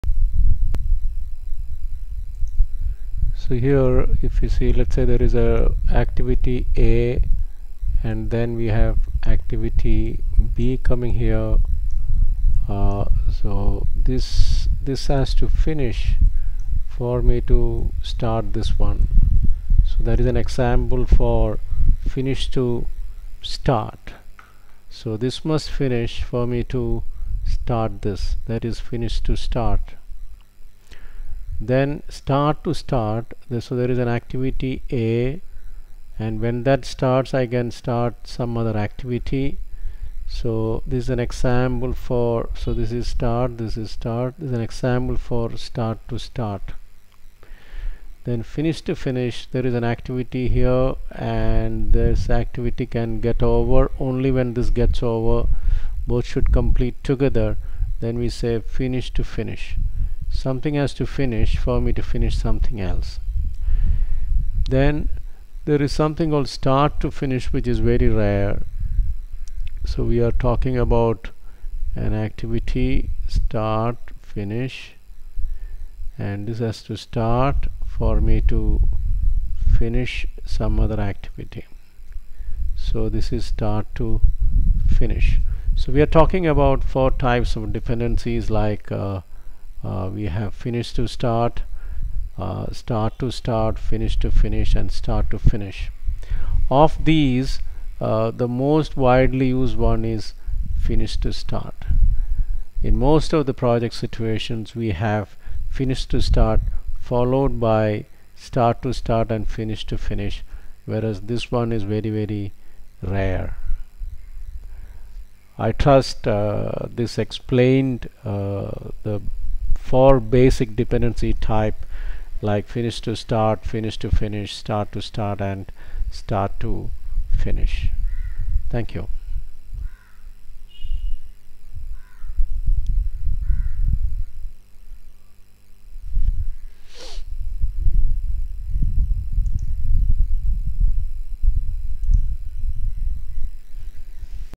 The four types of dependencies among tasks
Explains Finish to start, Start to start, Finish to finish and Start to Start dependencies